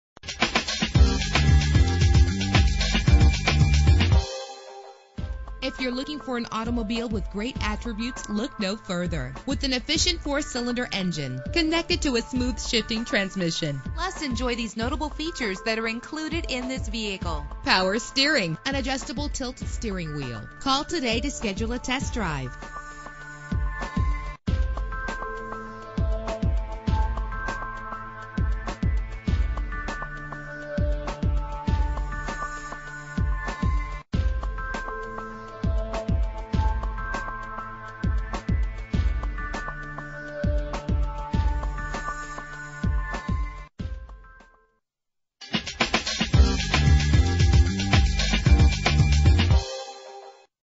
2000 Mitsubishi Mirage - Philadelphia PA
Year: 2000
Make: Mitsubishi
Model: Mirage
Trim: DE
Engine: 1.5 liter inline 4 cylinder 12 valve
Transmission: AIR CONDITIONING ALARM SYSTEM AM FM ANTI LOCK BRAKES BUCKET SEATS CD CRUISE CONTROL DRIVER AIRBAG PASSENGER AIRBAG POWER LOCKS POWER STEERING POWER WINDOWS REAR DEFROSTER TILT WHEEL
Color: Charcoal
Mileage: 43374
Address:
1501 Cottman Ave